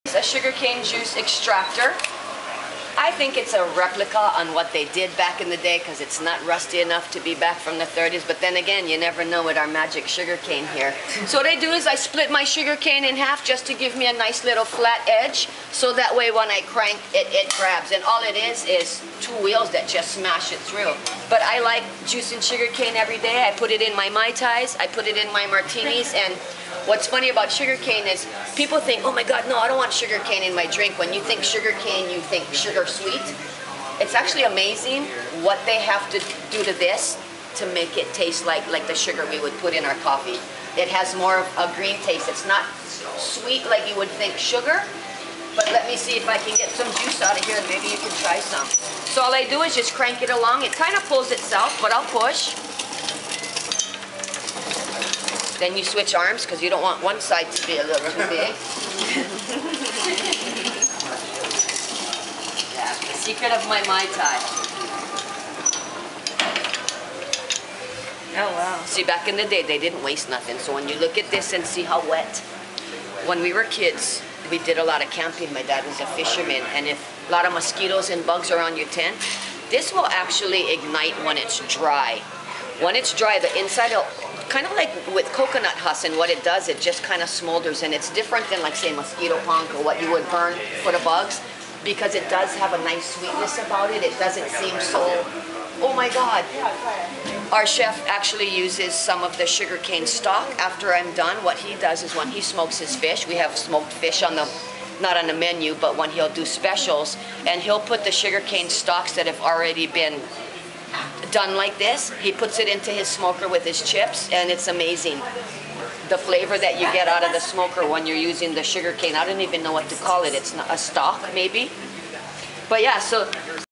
how a sugarcane juice extractor works
Kilohana Plantation, Lih'ue, Kauai, Hawaii
Shot by Carole Terwilliger Meyers on May 23, 2014
Laurie Shibao demonstrates a sugarcane juice extractor and explains how she makes the island's best mai tai.
c2014 Carole Terwilliger Meyers